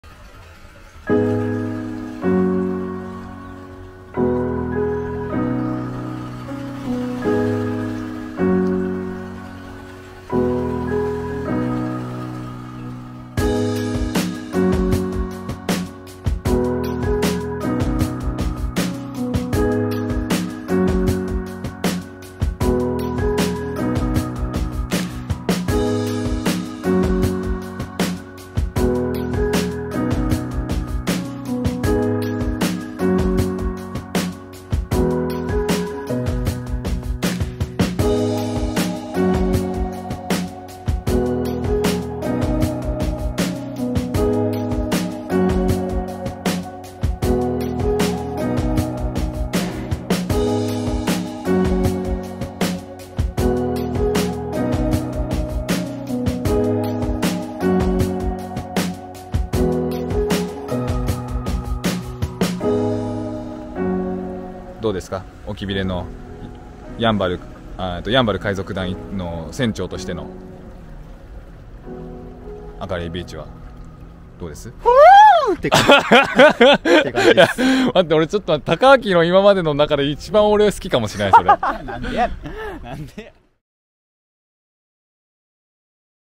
【オキナワビレッジ】東江海岸〜Agarie beach〜2021.9.20【ヤンバル海賊団VLOG】
沖縄県名護市にある人口ビーチ東江海岸のサンセットです。
すぐ後ろに市街地があるとは思えないくらいエモいです。

動画制作元：オキナワビレッジ
参加お待ちしています。